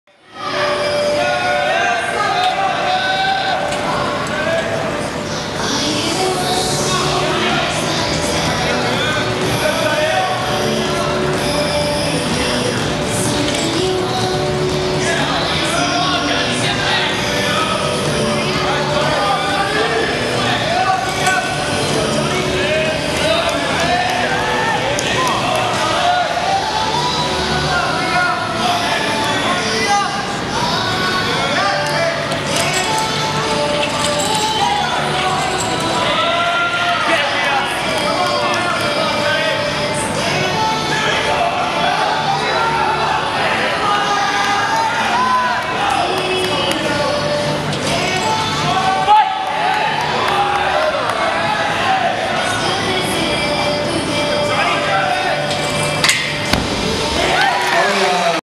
Navy Meet 2/21/2016- Aria Sabbagh R
Dual Meet vs. Navy on 2/21/2016. Aria Sabbath, Rings 13.8.